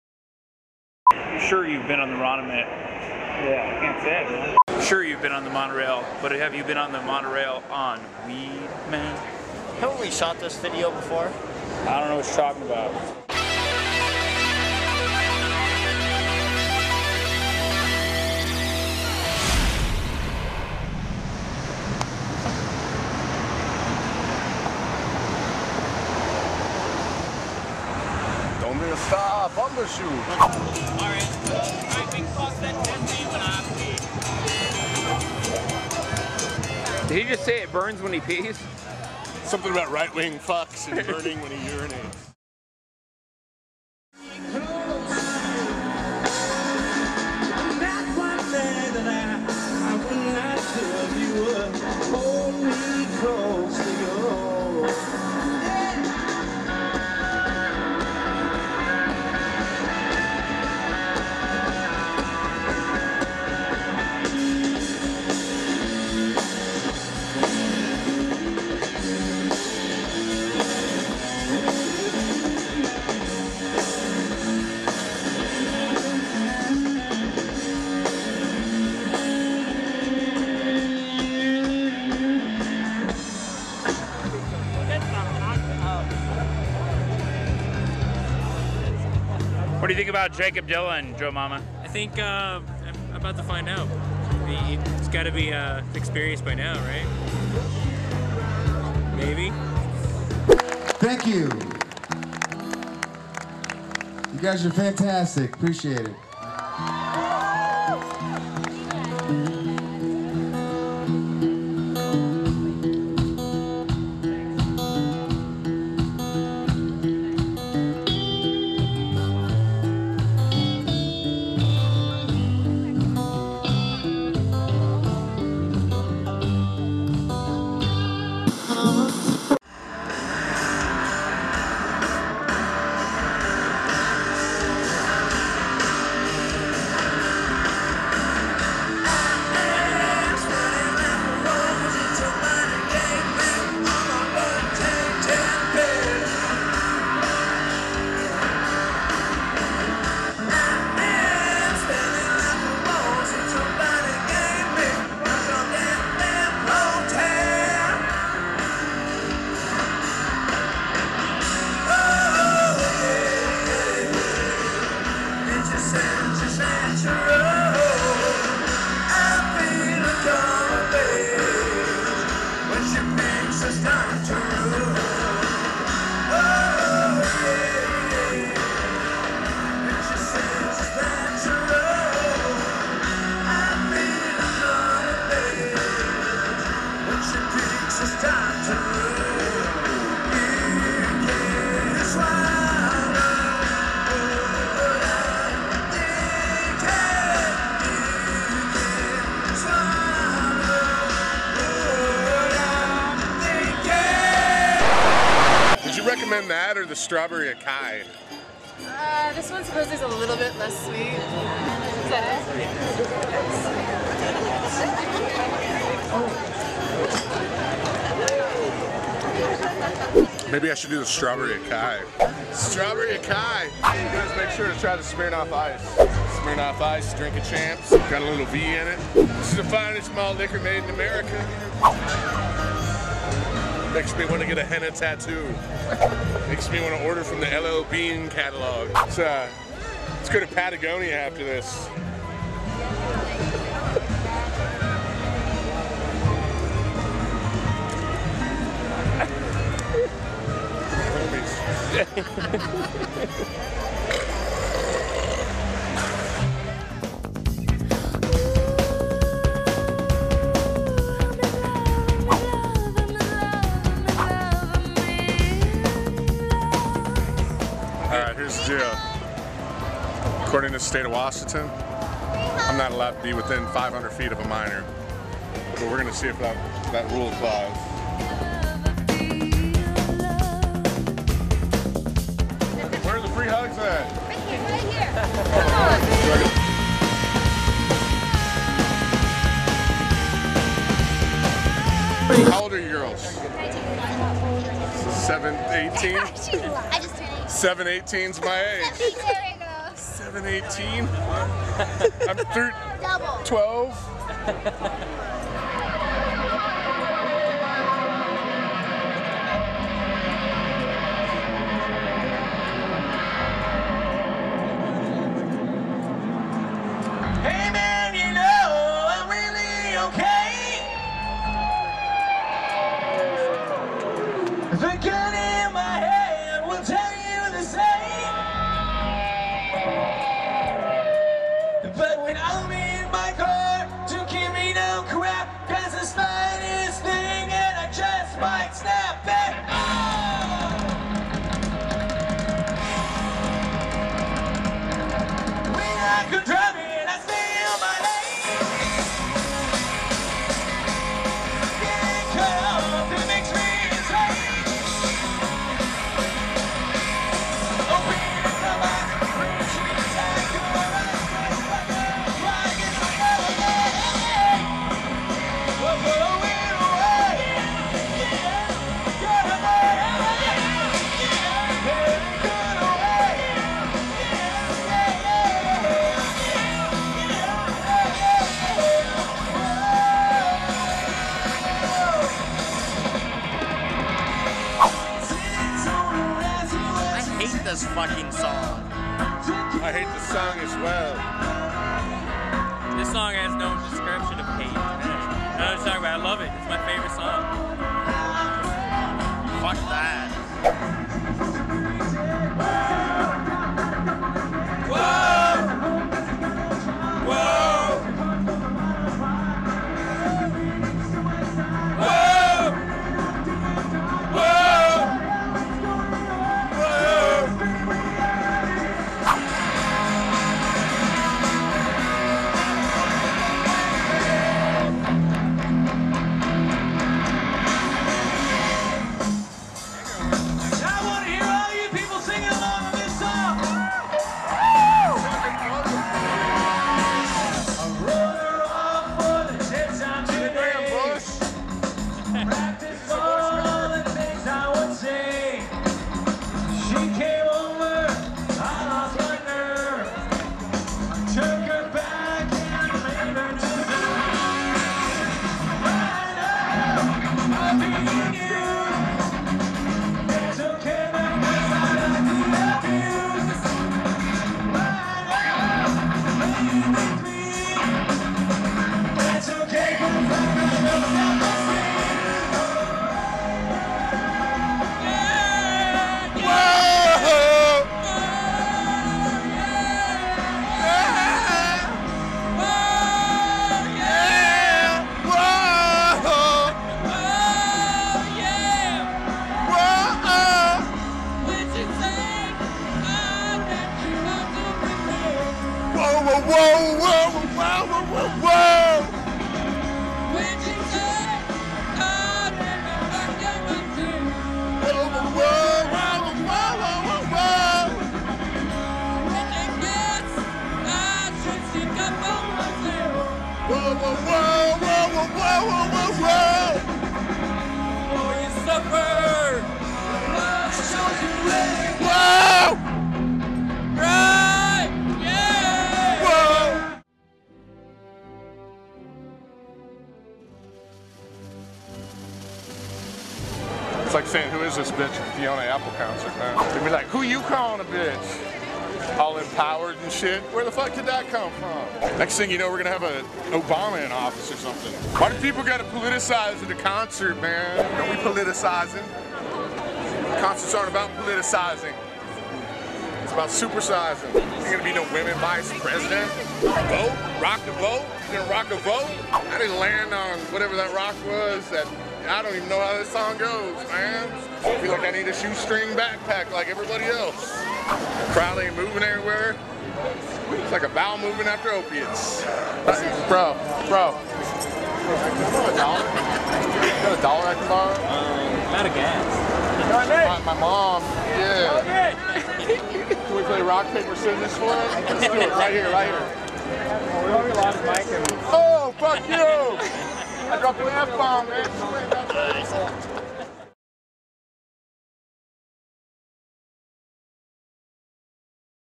Bumbershoot 2008 - Stone Temple Pilots, Offspring and Sarah Palin
Bumbershoot 2008 in Seattle Washington. Live concert footage of the offspring, jakob dylan, the black keys, stone temple pilots and politics of Sarah Palin Barack Obama.